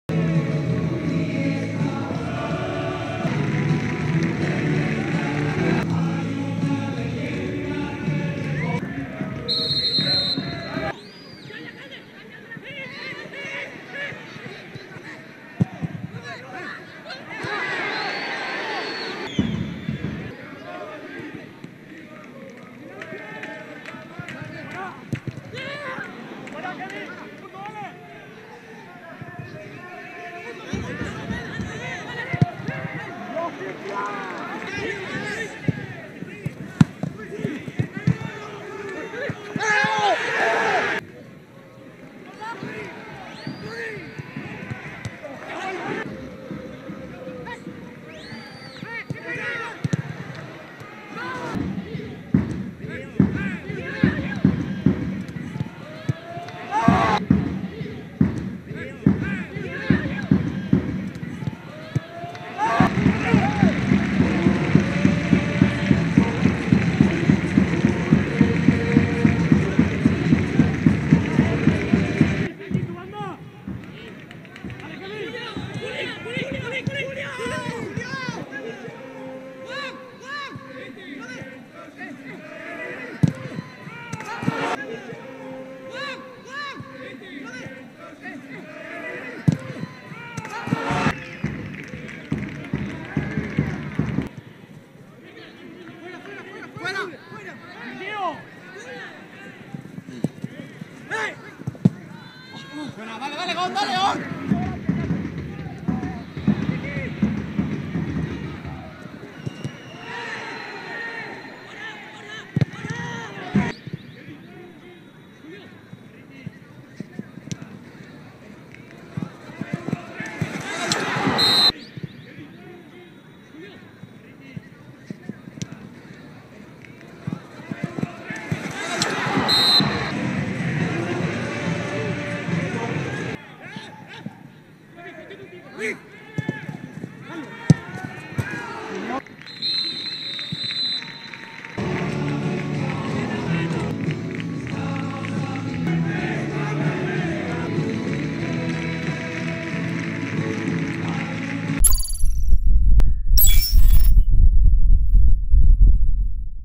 Resumen del partido Betis Deportivo-Xerez CD (3-0)
Resumen con las mejores imágenes y los goles del partido Betis Deportivo-Xerez CD, correspondiente a la jornada 23 en el Grupo 10 de Tercera División.